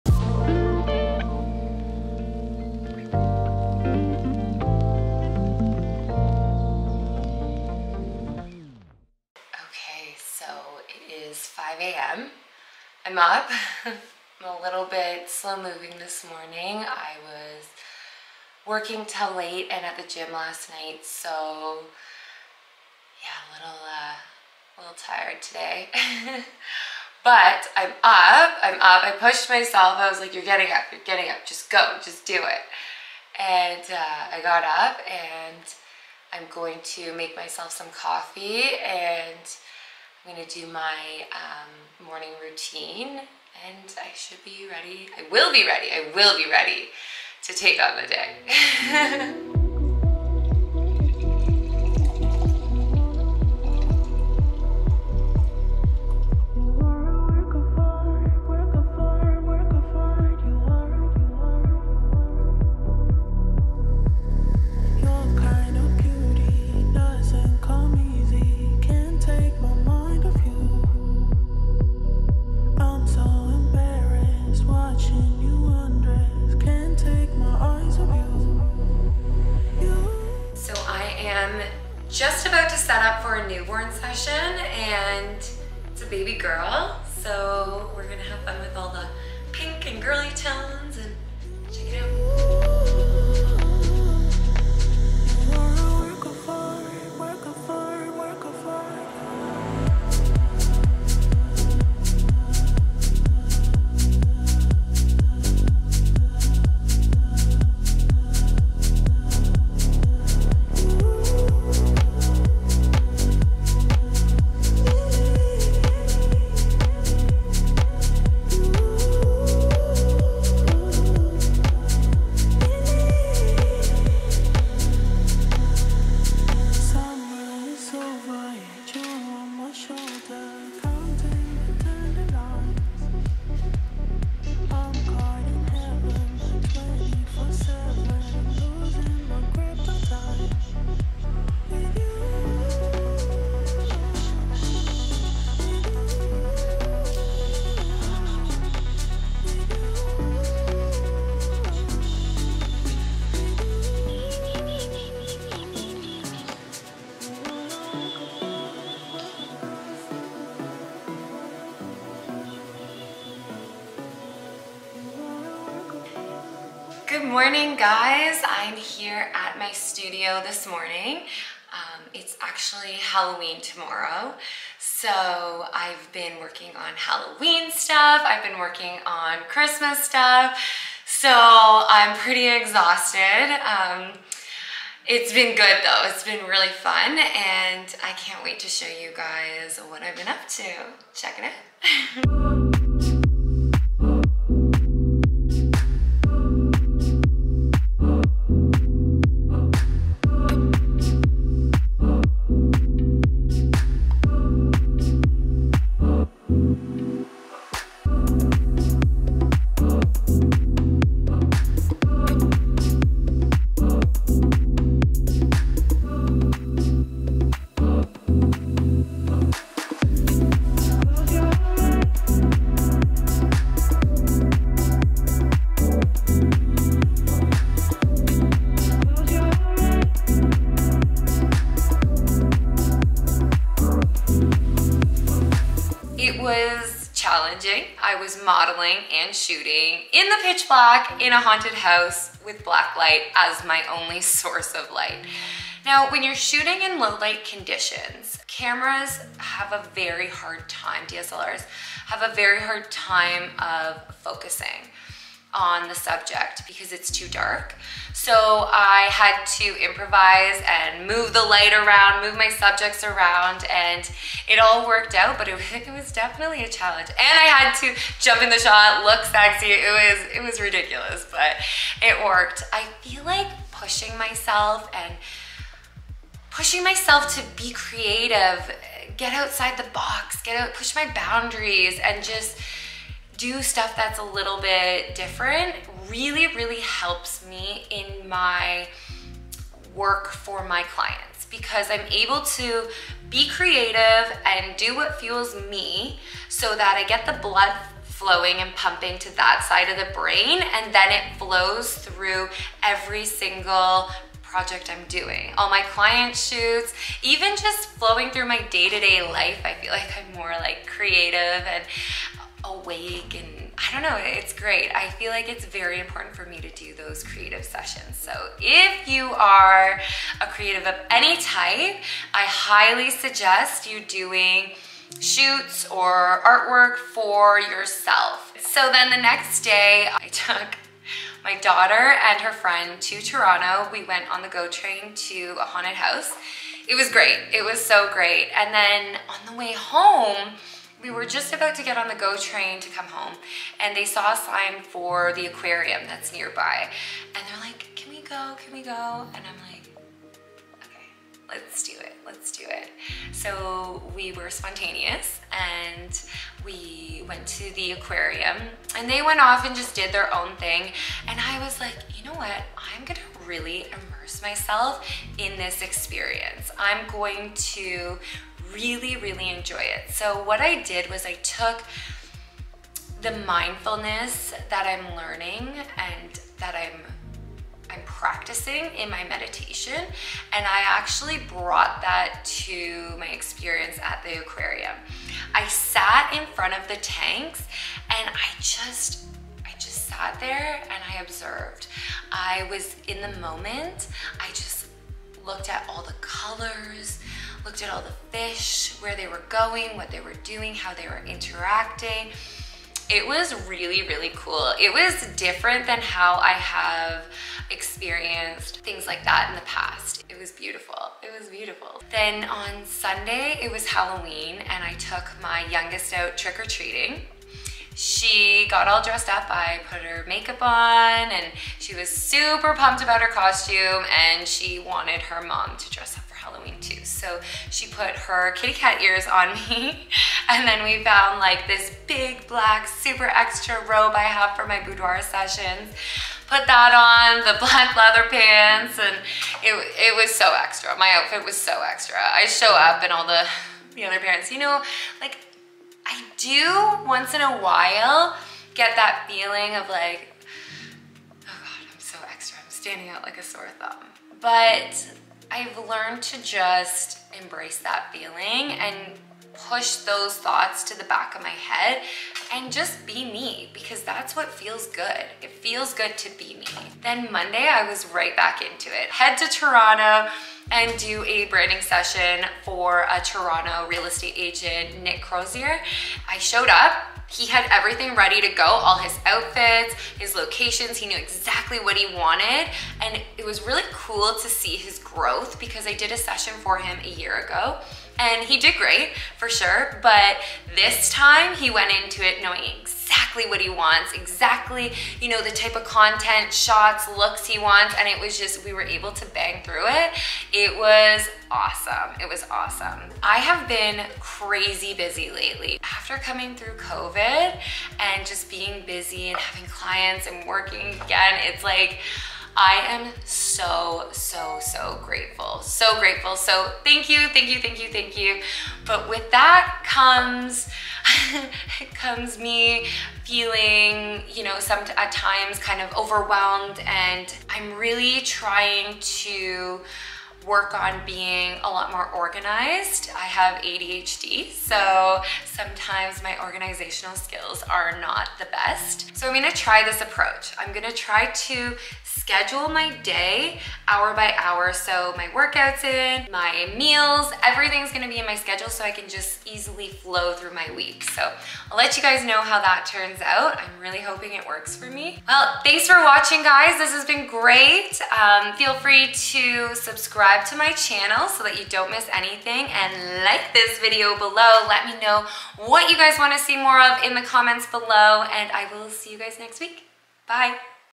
Sarah Martin Studios | Vlog Episode Three
In this vlog you will get a look into Sarah's life and what happens behind the scenes at Sarah Martin Studios. From babes to babies to branding... Sarah covers it all.